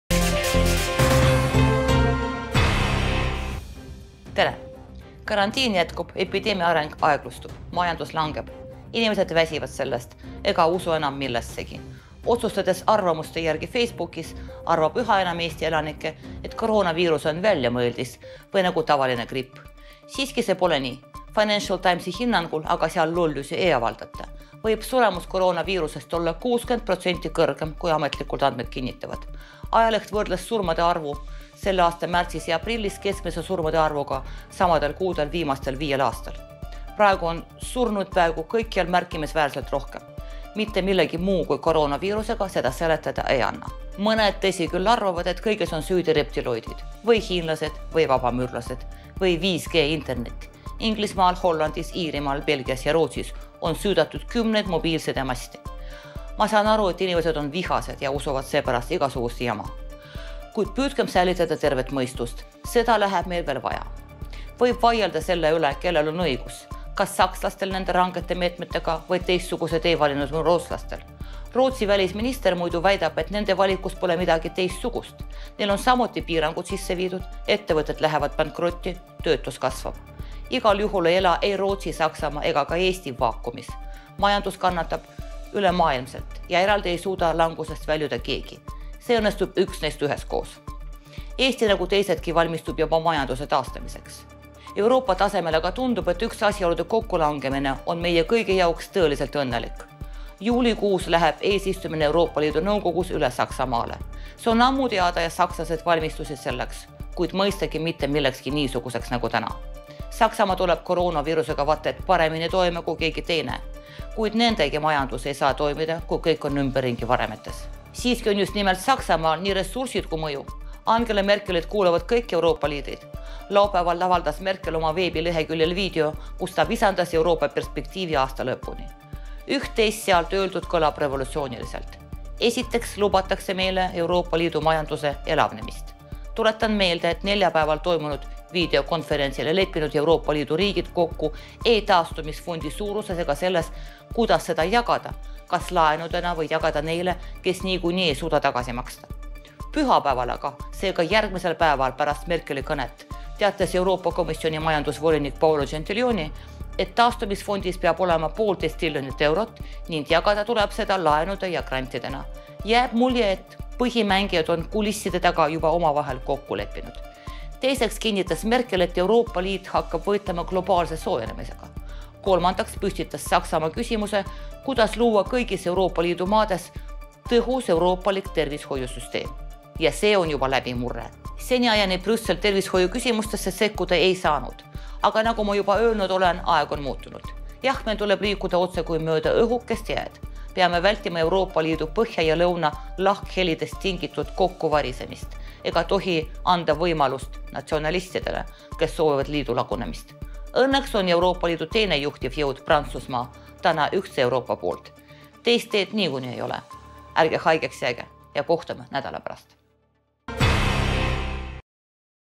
“Yana Toomi Brüsseli päevik”: koroonaviirus, Saksamaa ja EL-i elavnemnine
“Yana Toomi Brüsseli päevik”: Merkel soovib luua kõigis Euroopa Liidu maades tõhus euroopalik tervishoiusüsteem